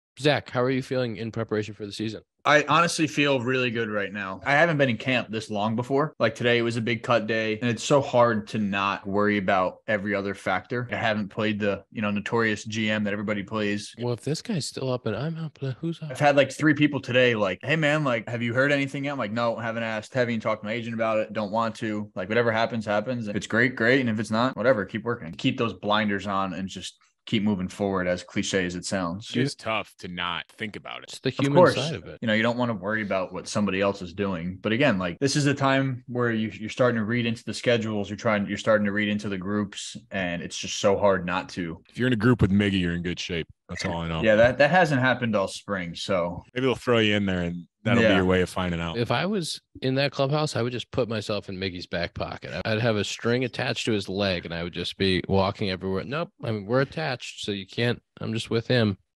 MLB player waiting to hear if he made the roster
Featuring: Zack Short, Dakota Mekkes, Ian Happ
Edited by: Max Mannis